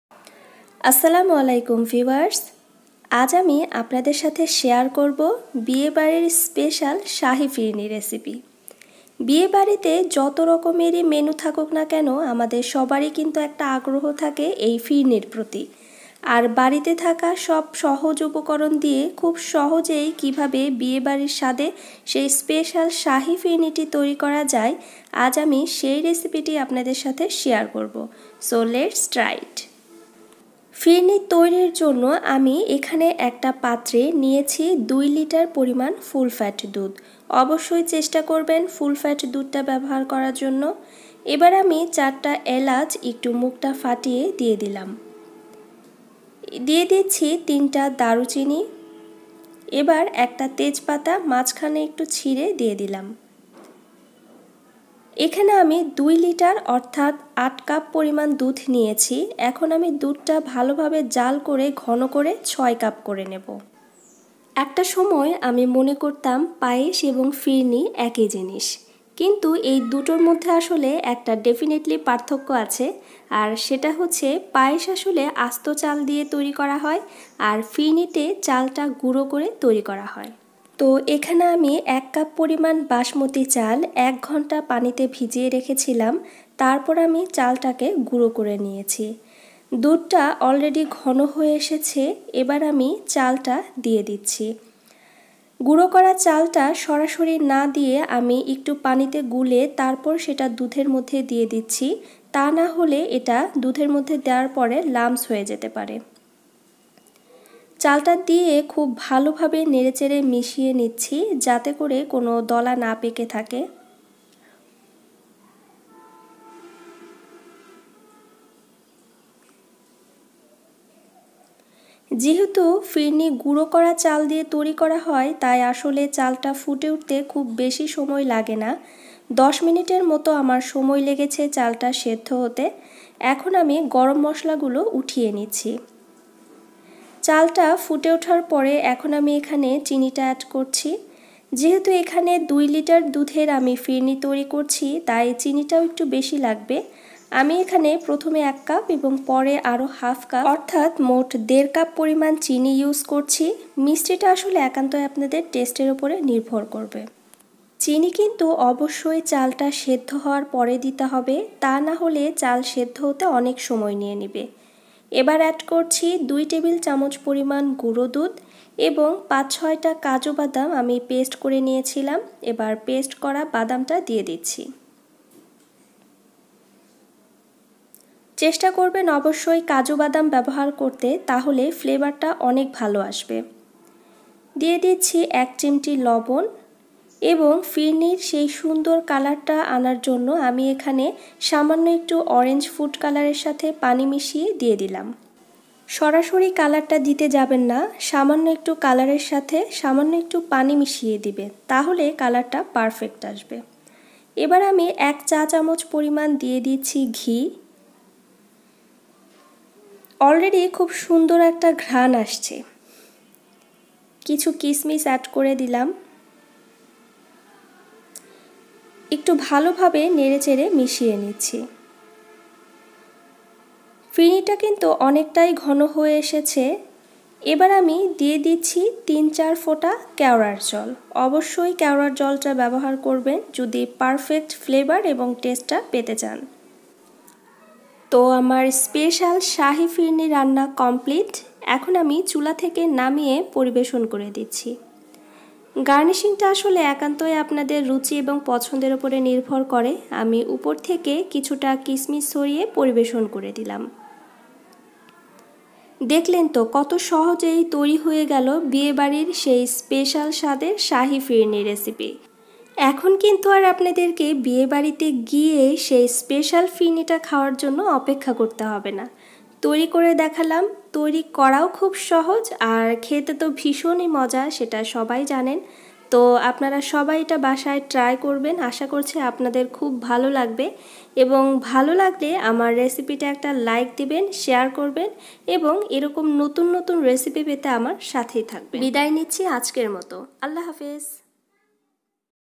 বিয়ে বাড়ির স্পেশাল শাহী ফিরনি//Perfect Sahi Phirni Recipe.
বিয়ে বাড়ীতে যত রকমের মেনুই থাকুক না কেন সবার আগ্রহ থাকে স্পেশাল শাহী ফিরনির প্রতি। তাই আজ আপনাদের সাথে শেয়ার করলাম বিয়ে বাড়ির স্পেশাল শাহী ফিরনির রেসিপি।এখন আর বিয়ে বাড়ির ফিরনির জন্য অপেক্ষা না করে বাড়িতে খুব সহজেই ঝটপট তৈরি করে ফেলুন দারুন মজার শাহী ফিরনি।